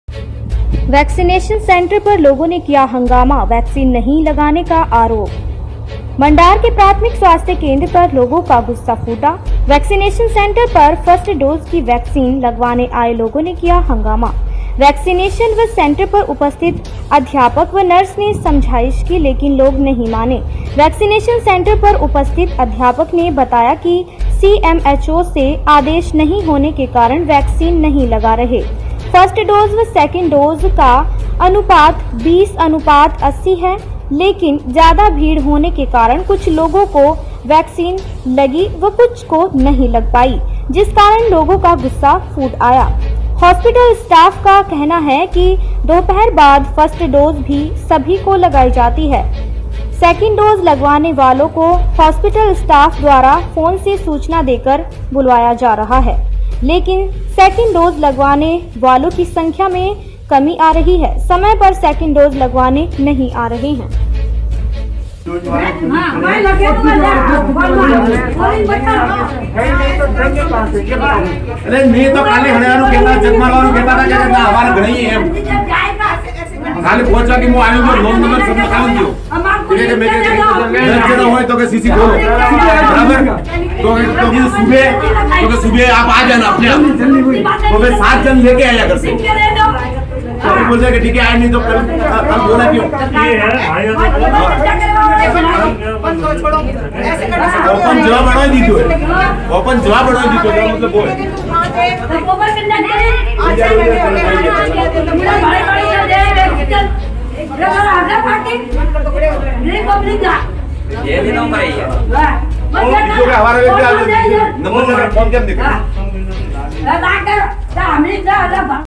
वैक्सीनेशन सेंटर पर लोगों ने किया हंगामा , वैक्सीन नहीं लगाने का आरोप ।
मंडार के प्राथमिक स्वास्थ्य केंद्र पर लोगों का गुस्सा फूटा , वैक्सीनेशन सेंटर पर फर्स्ट डोज की वैक्सीन लगवाने आए लोगों ने किया हंगामा , वैक्सीनेशन व सेंटर पर उपस्थित अध्यापक व नर्स ने समझाइश की लेकिन लोग नहीं माने वैक्सीनेशन सेंटर पर उपस्थित अध्यापक ने बताया कि CMHO से आदेश नहीं होने के कारण वैक्सीन  नहीं लगा रहे । फर्स्ट डोज व सेकंड डोज का अनुपात 20 अनुपात 80 है लेकिन ज्यादा भीड़ होने के कारण कुछ लोगों को वैक्सिंन लगी व कुछ को नहीं लग पाई जिस कारण लोगों का गुस्सा फूट आया । हॉस्पिटल स्टाफ का कहना है कि दोपहर बाद फर्स्ट डोज भी सभी को लगाई जाती है । सेकंड डोज लगवाने वालों को हॉस्पिटल स्टाफ द्वारा फोन से सूचना देकर बुलवाया जा रहा है , लेकिन सेकंड डोज लगवाने वालों की संख्या मैं कमी आ रही है समय पर सेकंड डोज लगवाने नहीं आ रहे ।

संवाददाता अल्ताफ सोढा , मंडार , सिरोही ।